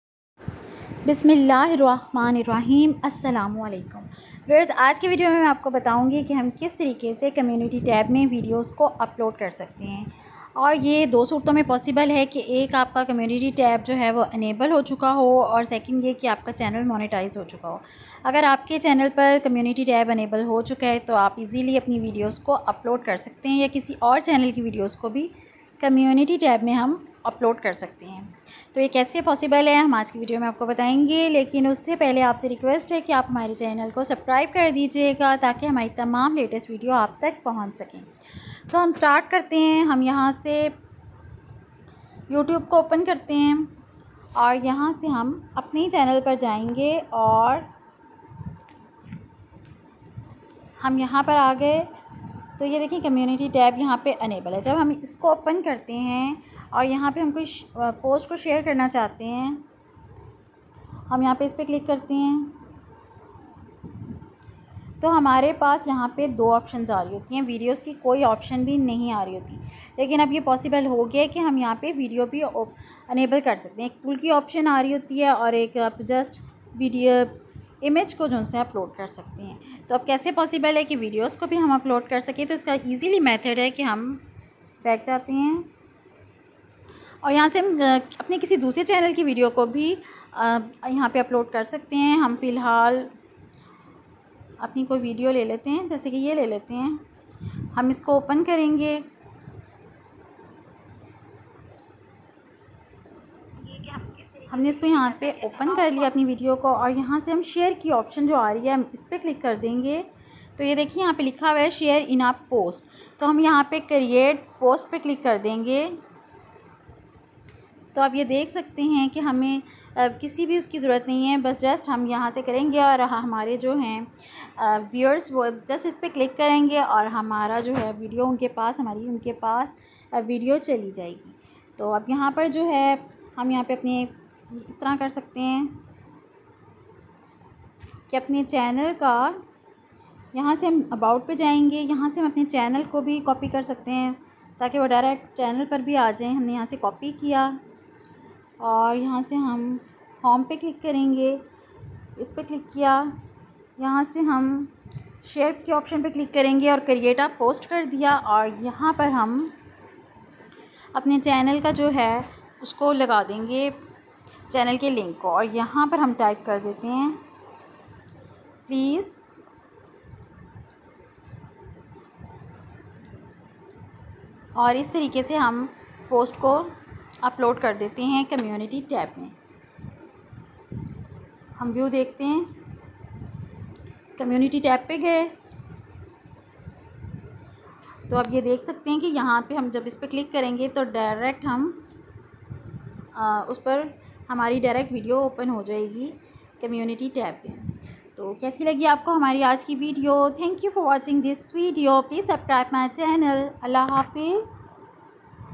How to upload videos on community tab
in this video I told you how to upload videos on community tab.